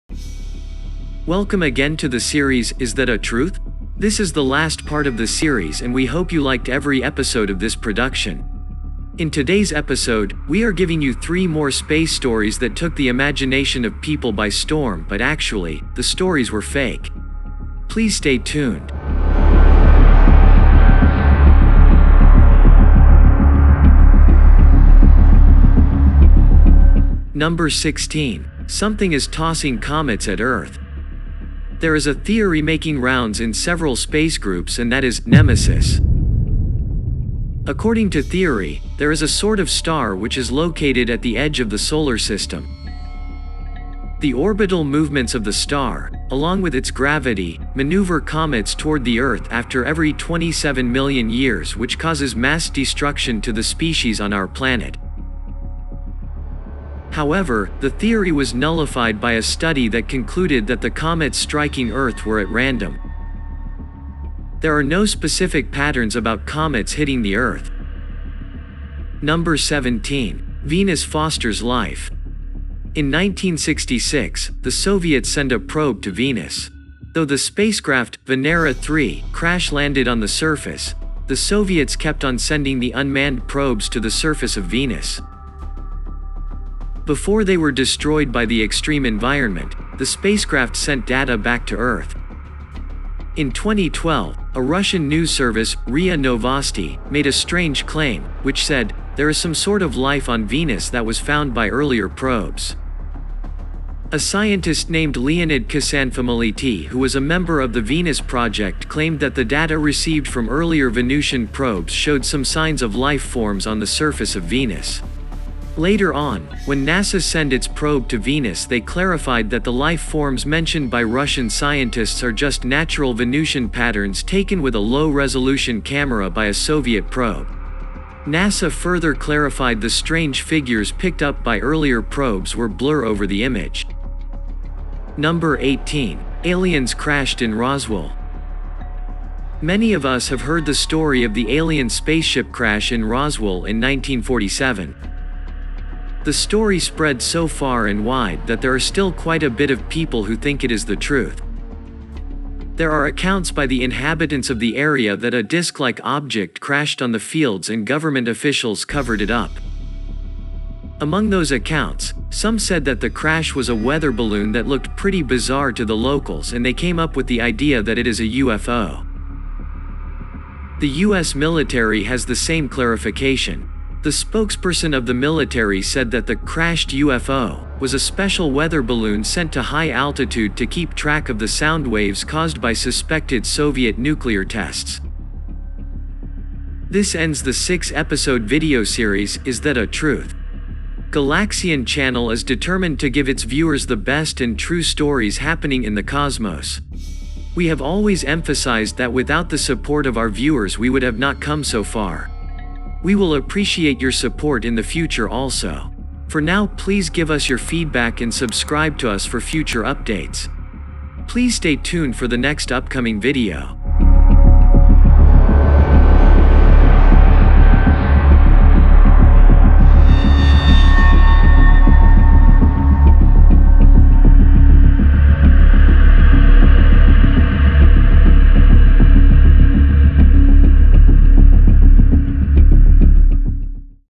Is That a Truth? Space Stories That Stormed the Imaginations of the People but are Fake. (PART 6)
Welcome again to the series "Is that a truth?". This is the last part of the series and we hope you liked every episode of this production. In today's episode, we are giving you 3 more space stories that took the imagination of people by storm but actually, the stories were fake.

#16. Something is tossing comets at earth.
There is a theory making rounds in several space groups and that is "Nemesis". According to theory, there is a sort of star which is located at the edge of the solar system. The orbital movements of the star, along with its gravity, maneuver comets toward the earth after every 27 million years which causes mass destruction to the species on our planet.

However, the theory was nullified by a study that concluded that the comets striking earth were at random. There are no specific patterns about comets hitting the earth.

#17. Venus fosters life.
In 1966, the Soviets send a probe to Venus. Though the spacecraft "Venera 3" crash landed on the surface, the Soviets kept on sending the unmanned probes to the surface of Venus.
Before they were destroyed by the extreme environment, the spacecraft sent data back to earth.
In 2012, a Russian news service "RIA Novosti" made a strange claim, which said, "there is some sort of life on Venus that was found by earlier probes." A scientist named Leonid Ksanfomaliti who was a member of the Venus project claimed that the data received from earlier Venusian probes showed some signs of life forms on the surface of Venus.
Later on, when NASA send its probe to Venus they clarified that the life forms mentioned by Russian scientists are just natural Venusian patterns taken with a low-resolution camera by a Soviet probe. NASA further clarified the strange figures picked up by earlier probes were blur over the image.

#18. Aliens crashed in Roswell.
Many of us have heard the story of the Alien spaceship crash in Roswell in 1947. The story spread so far and wide that there are still quite a bit of people who think it is the truth. There are accounts by the inhabitants of the area that a disc-like object crashed on the fields and government officials covered it up.
Among those accounts, some said that the crash was a weather balloon that looked pretty bizarre to the locals and they came up with the idea that it is a UFO.
The US military has the same clarification. The spokesperson of the military said that the "Crashed UFO" was a special weather balloon sent to high altitude to keep track of the sound waves caused by suspected Soviet nuclear tests.